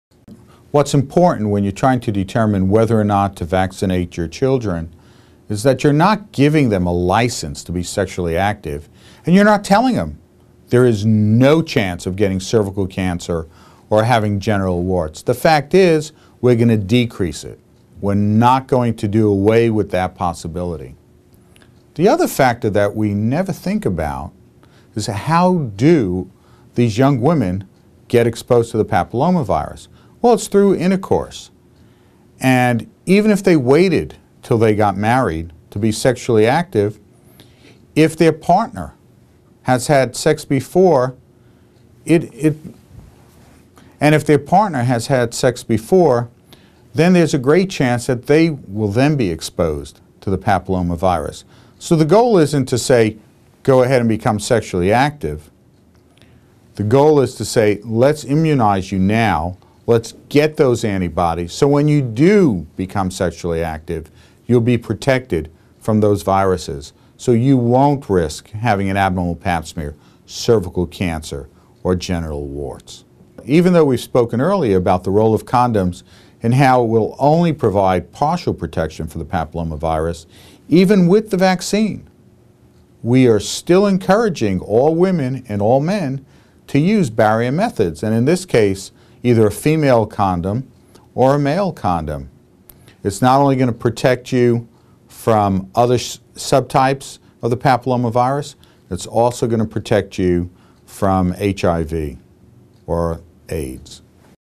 14-Child Vaccination - Interview with Dr. Ira Horowitz
Dr. Ira Horowitz is Willaford Ransom Leach Professor in Gynecologic Oncology, Vice Chair for Clinical Affairs, Chief Medical Officer, Emory University Hospital, Associate Director, The Emory Clinic. Dr. Horowitz is a gynecologist and an expert in cervical cancer. In this interview he discusses the prevention, cause and treatment of cervical cancer. The human papillomavirus (HPV) is a causative agent of cervical cancer and Dr. Horowitz explains how it is transmitted and how transmission can be reduced.

In this interview segment, Dr. Horowitz discusses if parents should get their children vaccinated for HPV.